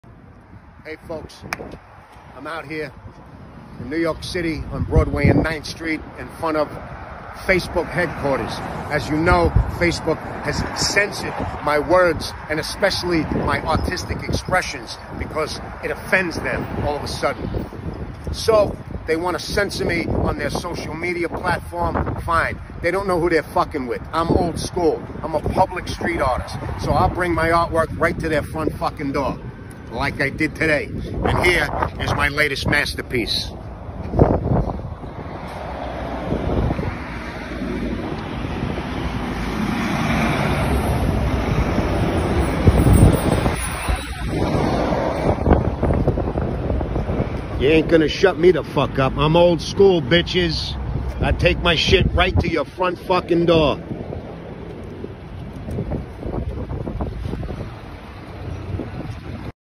Free Speech | Artistic Expression - By Scott LoBaido
“Free Speech”
[Scott LoBaido’s Artistic Expression]

After being censored on Facebook, Scott LoBaido takes action!
On Wednesday, February 24th (2021), Scott LoBaido decided to take a stand and *ARTISTICALLY* expressed himself in front of Facebook Headquarters in New York.
No violence, just a bold statement in the form of *STREET ART* that Facebook won’t be able to miss or ignore.
They can *TRY* to silence us on their platform(s), but that won’t make us go away, remain silent and they most definitely won’t force us into submission.
Scott LoBaido is an old-school street artist who keeps it 100%!
Hope you enjoy his masterpiece!